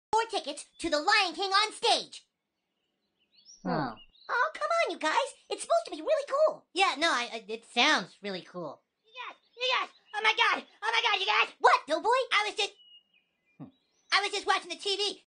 “Dough boy” South Park animation
I can’t believe I never posted this on YouTube
It’s very old and lowkey shitty but this clip still cracks me up